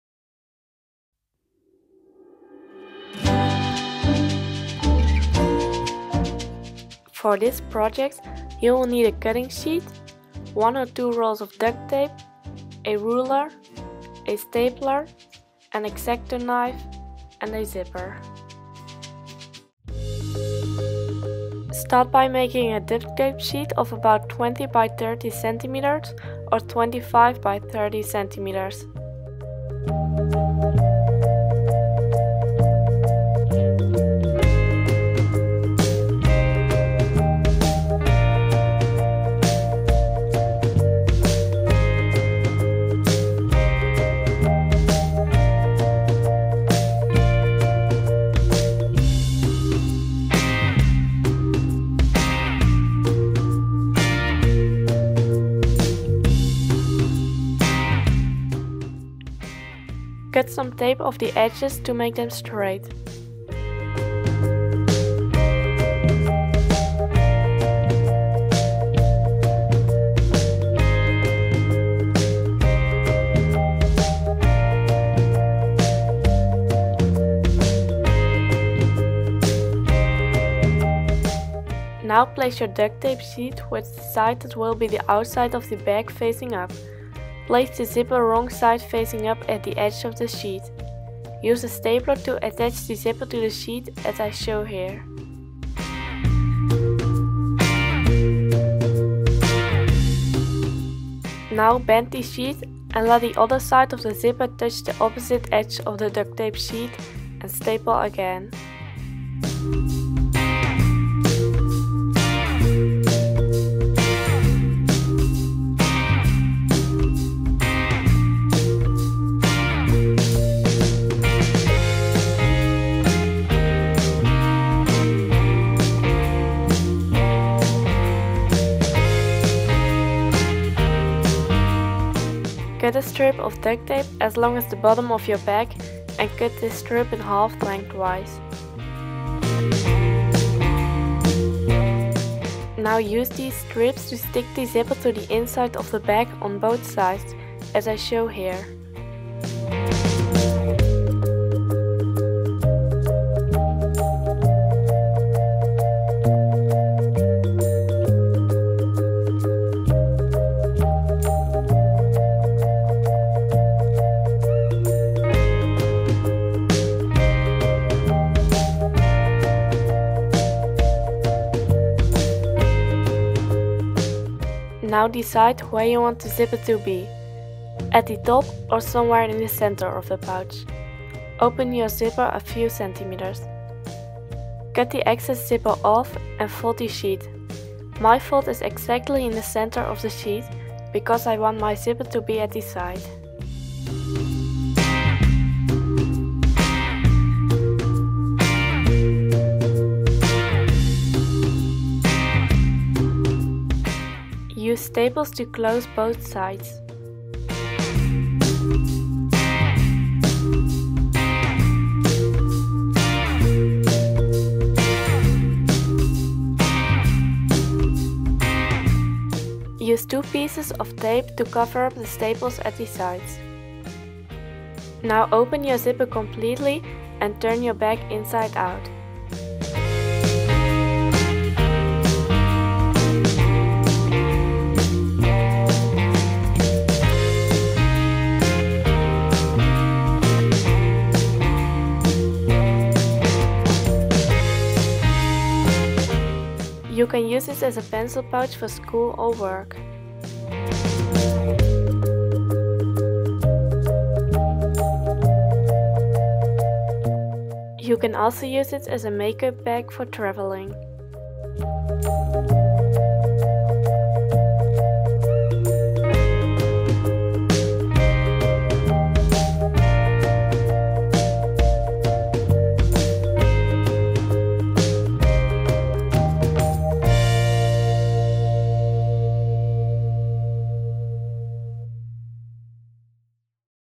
DIY Duct Tape Pencil/Makeup Bag
Open the description box to read everything you need to know.
Open de beschrijving om alles te lezen wat je moet weten.

In this video I show you how you can make a small bag with duct tape that you can use for school/work supplies or makeup. This bag is easy to customize, you can use many kinds of duct tape and vary in size.
You can find a lot of colored duct tapes at this

Necessities:
- cutting sheet
- duct tape (1 or 2 rolls)
- ruler
- stapler
- x-acto knife
- zipper

Share your recreation with me on Instagram or Twitter (@guineafleur)!
If you like this video, please share it and give a thumb up!

In deze video laat ik zien hoe je een etui kan maken die je kan gebruiken voor je school- of werkspullen of make-up. Deze etui is makkelijk aan te passen aan je eigen smaak, je kan veel verschillende soorten duct tape gebruiken en variëren in grootte.

Benodigdheden:
- snijmat
- duct tape (1 of 2 rollen)
- liniaal
- nietmachine
- mesje
- rits

Muziek: Youtube Audiolibrary